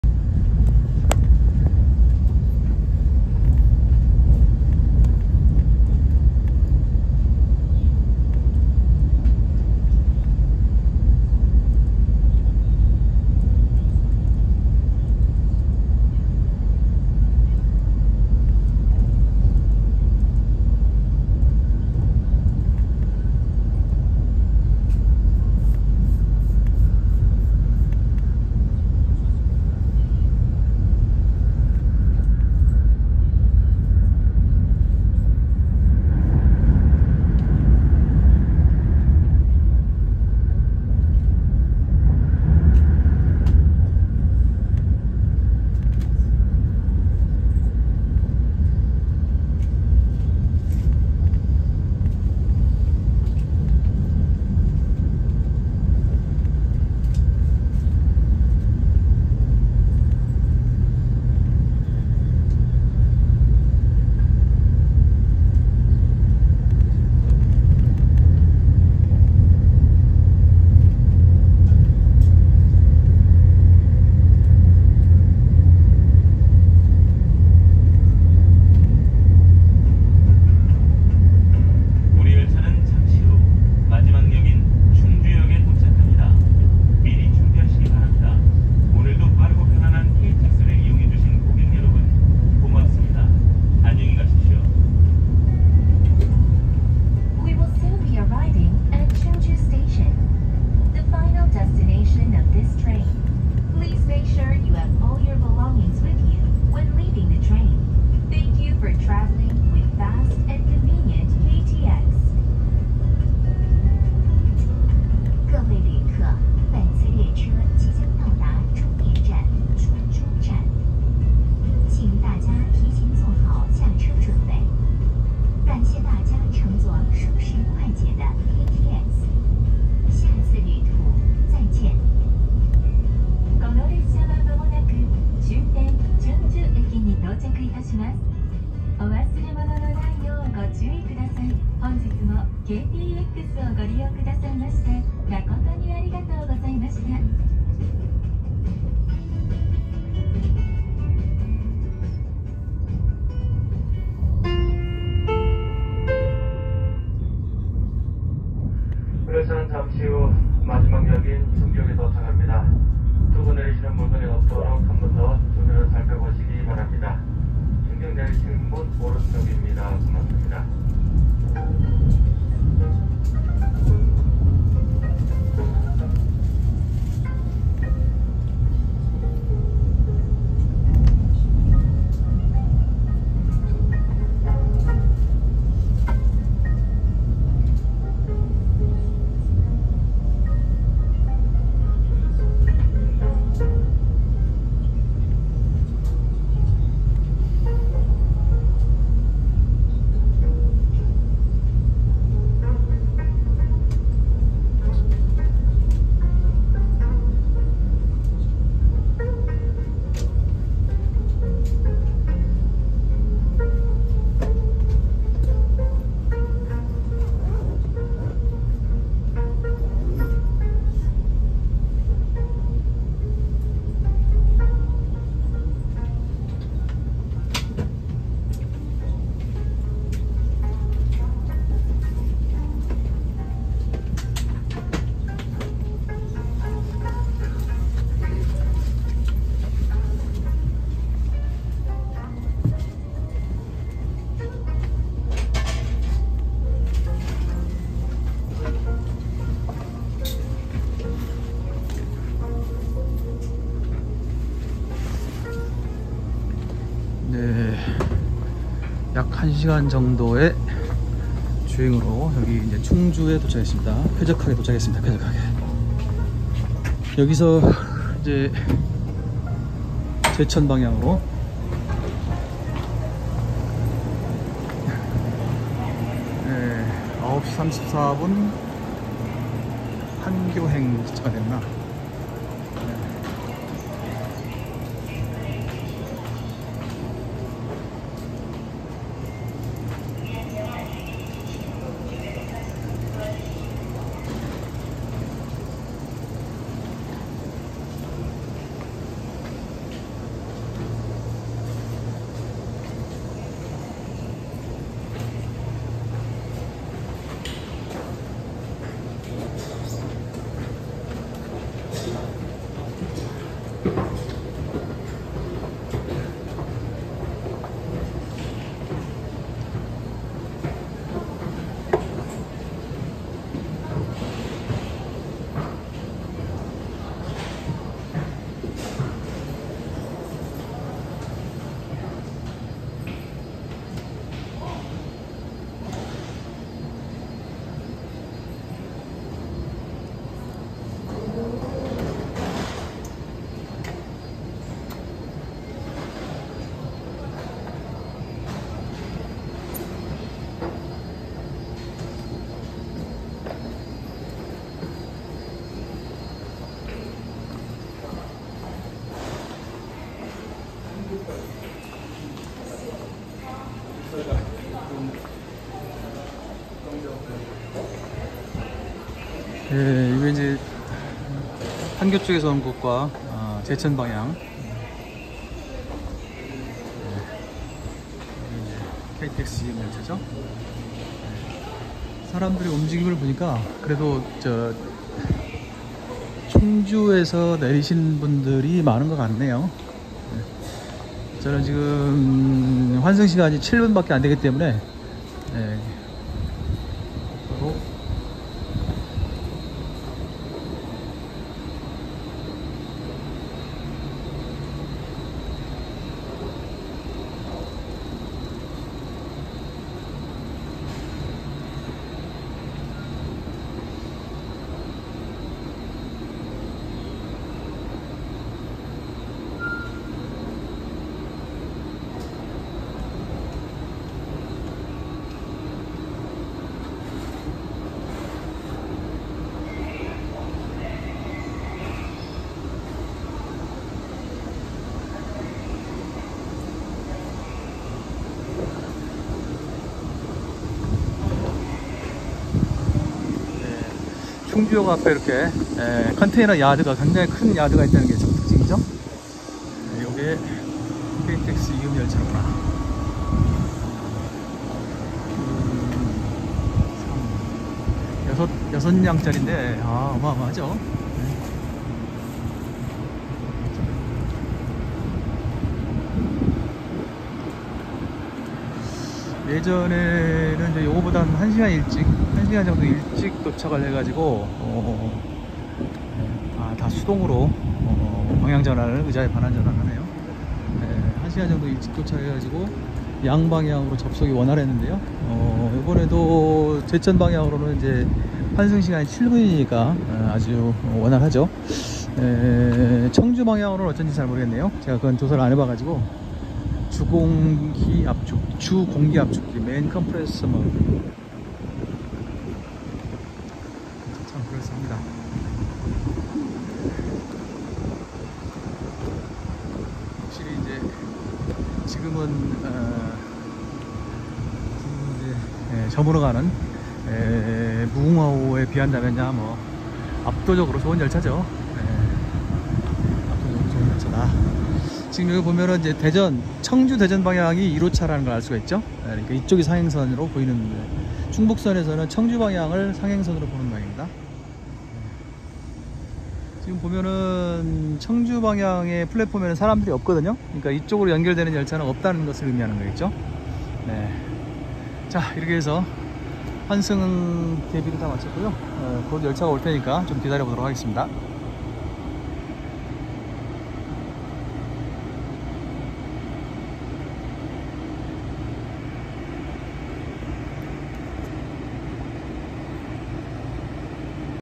(4)(충북경북선) 충주역 도착, 7분 환승시간, 충북선으로
남한강을 건넌 뒤 충북선과 병주한 다음 충주역에 도착합니다. 환승시간이 7분밖에 되지 않으므로 서둘러 제천행 플랫폼으로 이동합니다.